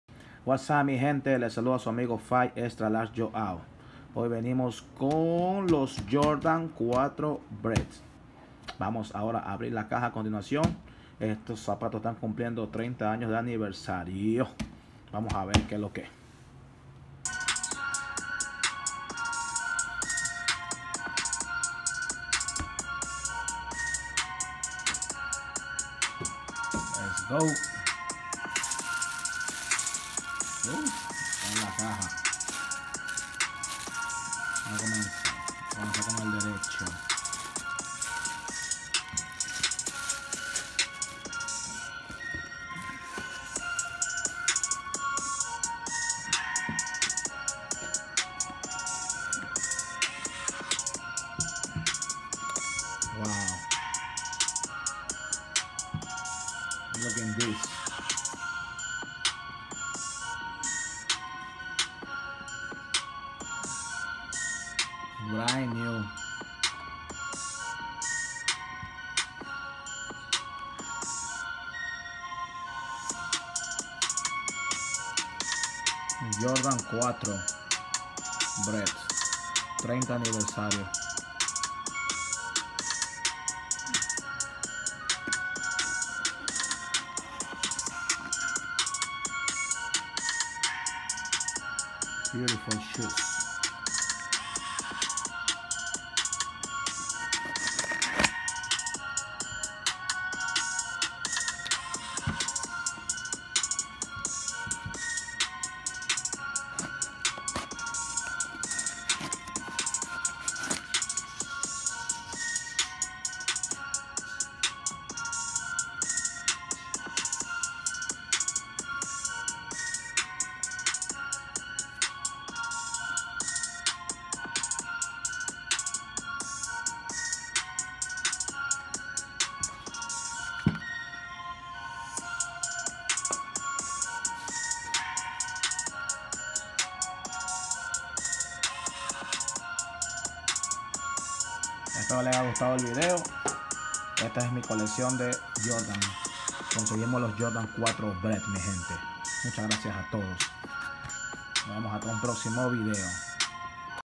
Jordan retro 4 bred para la coleccion
Los zapatos los compre en food locker mi gente buen precio